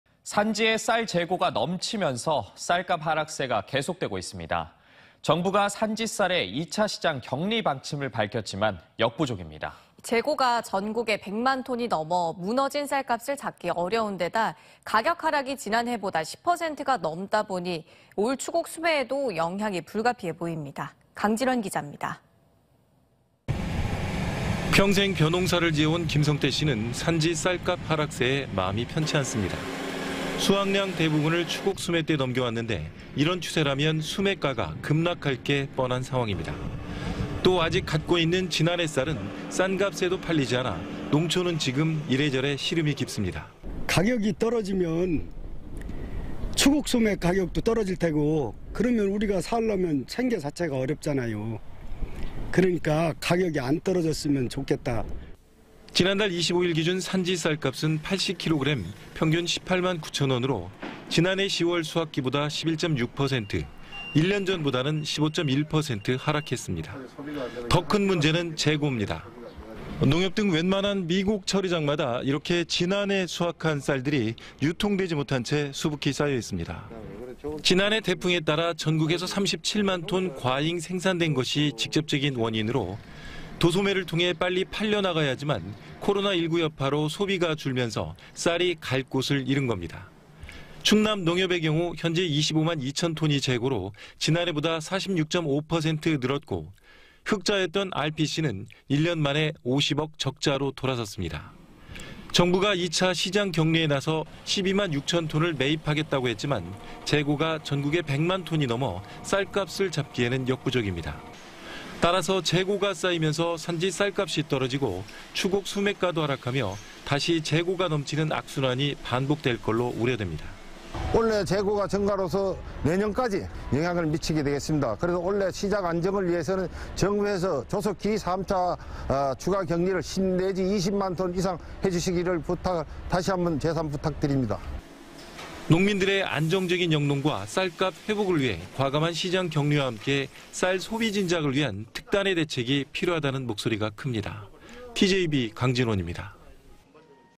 쌀값 하락..넘치는 재고 어쩌나｜ TJB 대전·세종·충남뉴스
【 앵커멘트 】

산지에 쌀 재고가
넘치면서
쌀값 하락세가

정부가
산지 쌀의
2차 시장 격리 방침을 밝혔지만
역부족입니다.

재고가 전국에 100만톤이 넘어
무너진 쌀값을 잡기 어려운데다
가격하락이 지난해보다
10%가 넘다 보니
올 추곡 수매에도
영향이 불가피해 보입니다.

강진원 기자.